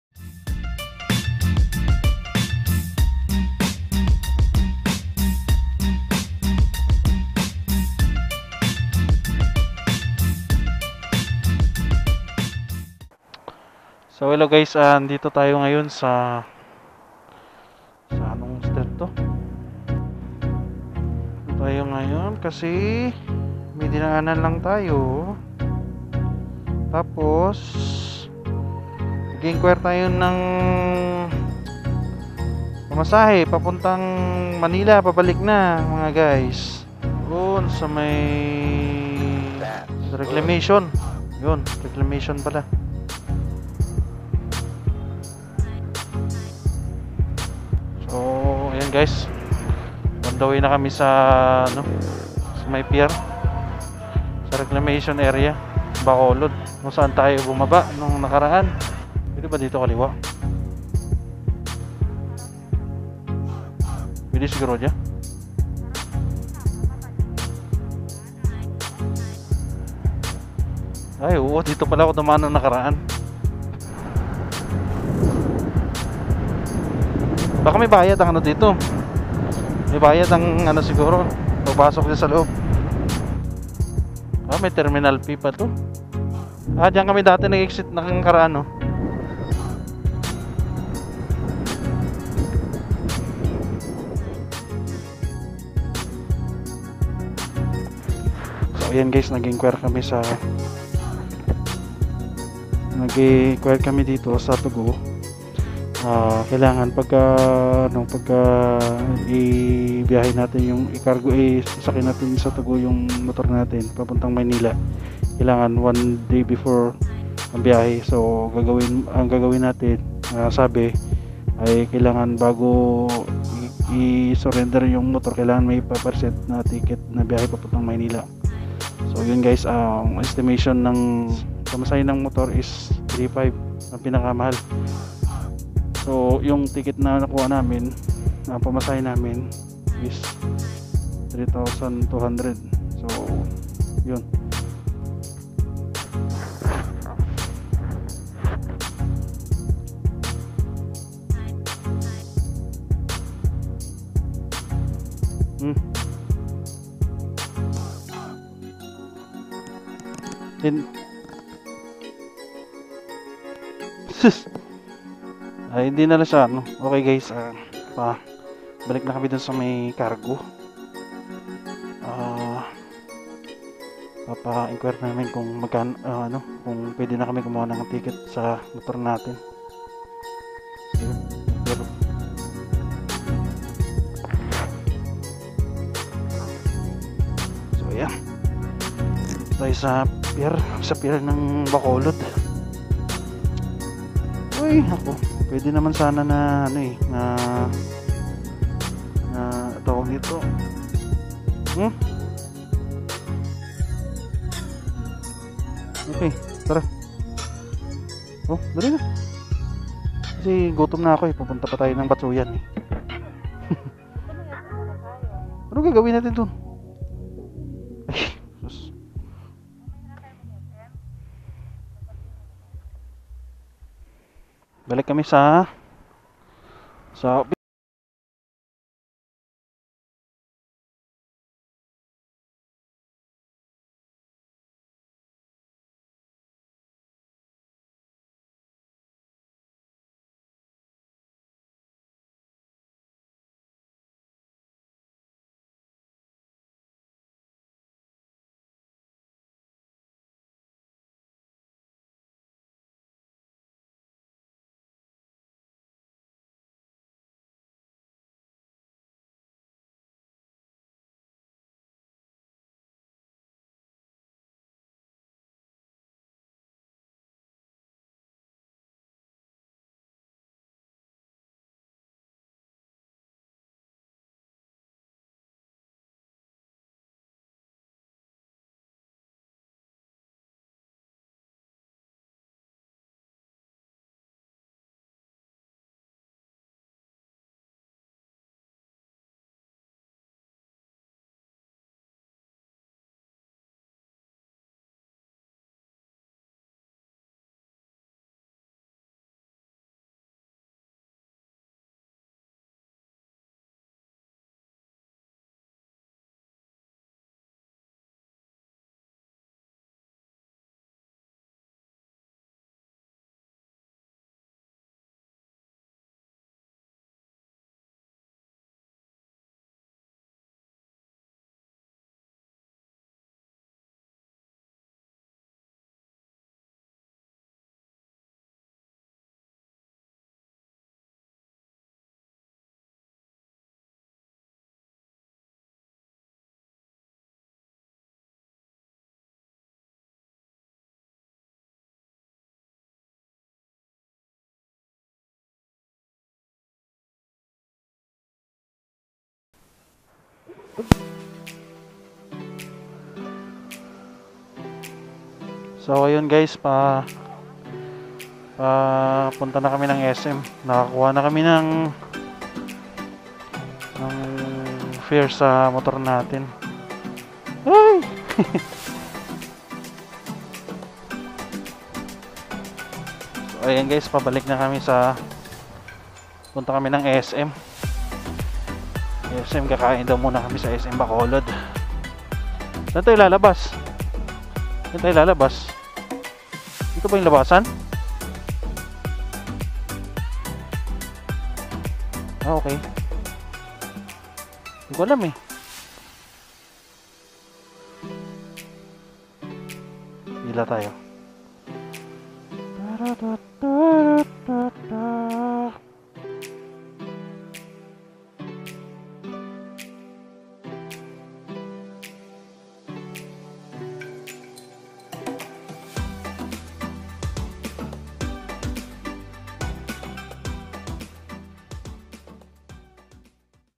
Bacolod to manila 2go fare with motorcycle magkano ba?
wassap mga guys eto na nag inquiry kami kung magkano ang pamasahe sa 2go at agad nag pabook na kami ni kumander para ma process na yung mga papers ni cb150r para less hustle, pag departure

( TAKE NOTE )

*kung plano mo isabay ung motor mo pag arrive sa manila dapat 1day before i surrender mo sa 2go ung motor mo.
kasi sila ang mag sasampa at mag kamada sa loon ng 2go at siguro delekado maraming container at truck na sasampa kaya sila ng bahala doon👍.

*bukod ang fare mo at ung motor mo.
depende ung fare mas maganda mag book ng early.

*ung fare ng motor depende sa laki at haba estemated P3,500K to 4,000k.

*dapat may ticket ka muna bago ka pumunta sa 2go cargo para ma process na ang mga documents.para sa araw ng pag surrender sa motor mo magbabayad ka na lang🙂.

gudluck sa byahe nyo ! 👍